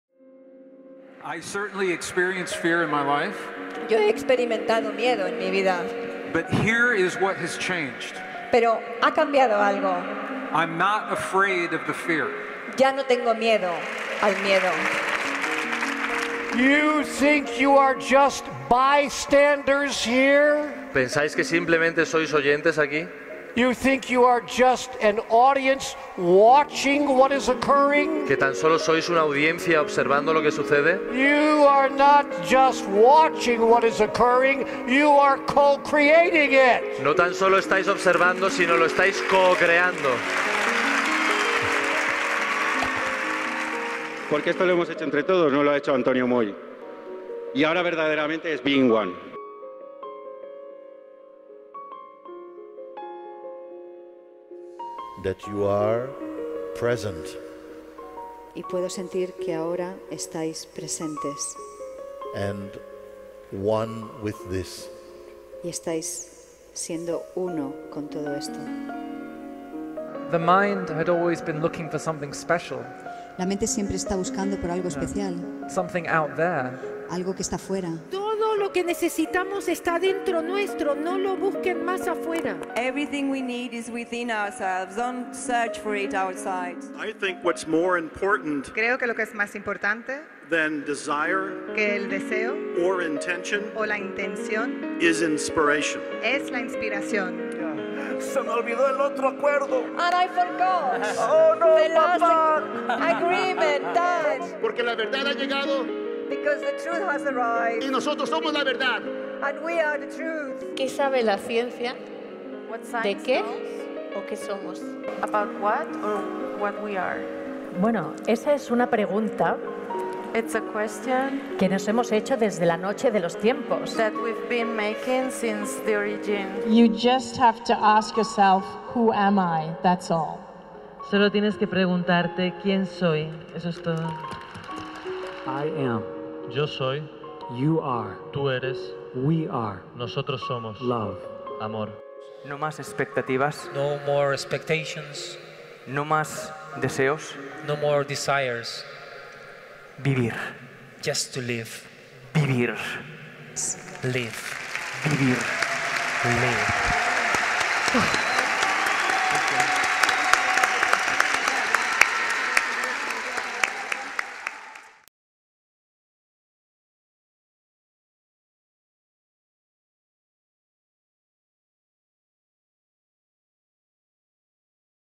ANTONIO MOLL: Being One, el mayor evento de desarrollo personal
Contenidos inéditos en un congreso trepidante que ha marcado un antes y un después en la historia del crecimiento personal y espiritual en Europa. Antonio Moll, Neale Donald Walsch, Gregg Braden, Mooji, Jeff Foster, Mabel Katz, Don Miguel Ruíz, Anita Moorjani, Joe Vitale y mucho más.